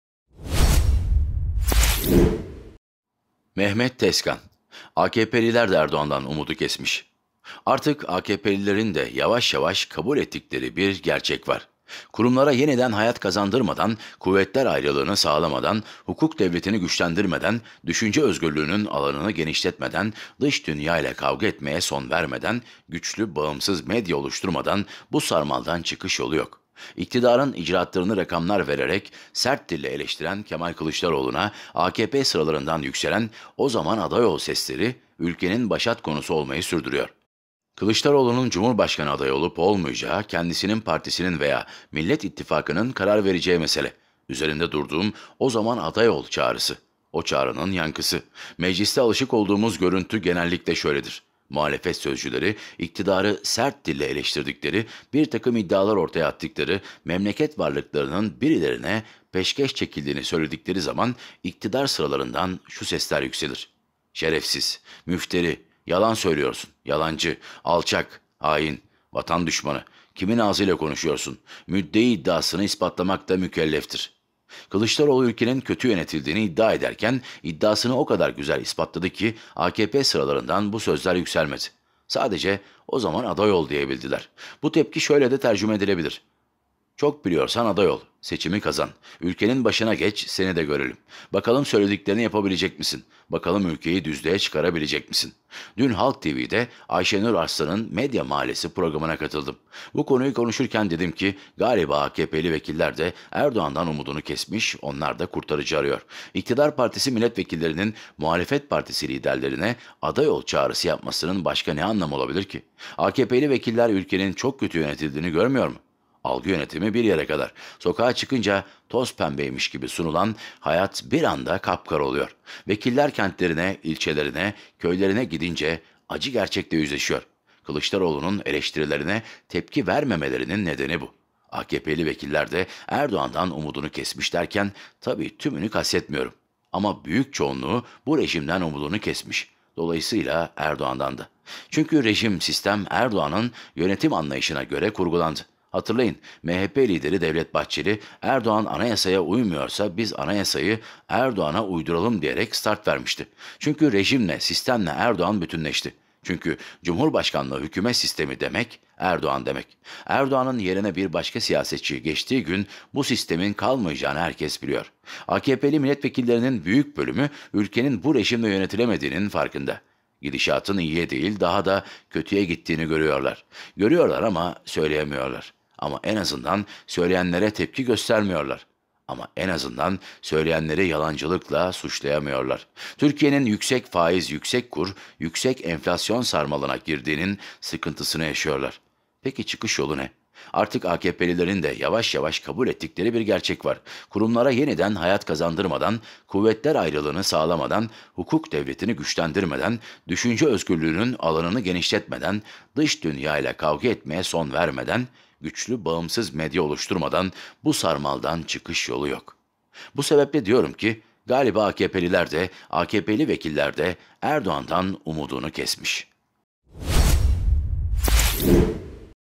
AKP'LİLER DE ERDOĞAN'DAN UMUDU KESMİŞ!
Artık AKP'lilerin de yavaş yavaş kabul ettikleri bir gerçek var: Kurumlara yeniden hayat kazandırmadan, kuvvetler ayrılığını sağlamadan, hukuk devletini güçlendirmeden, düşünce özgürlüğünün alanını genişletmeden, dış dünyayla kavga etmeye son vermeden, güçlü/bağımsız medya oluşturmadan bu sarmaldan çıkış yolu yok.

Youtube Koordinatörü : Havva Köse
Yönetmen: Mustafa Kuloğlu
Ses: Fatih Bar
Kamera: Onur Bingöl
Kurgu: Hakan Müstecep
Kurgu: Yusuf Özışık
İçerik Editörü: Ceren Ersöz
İçerik Editörü: Asel Uğurlu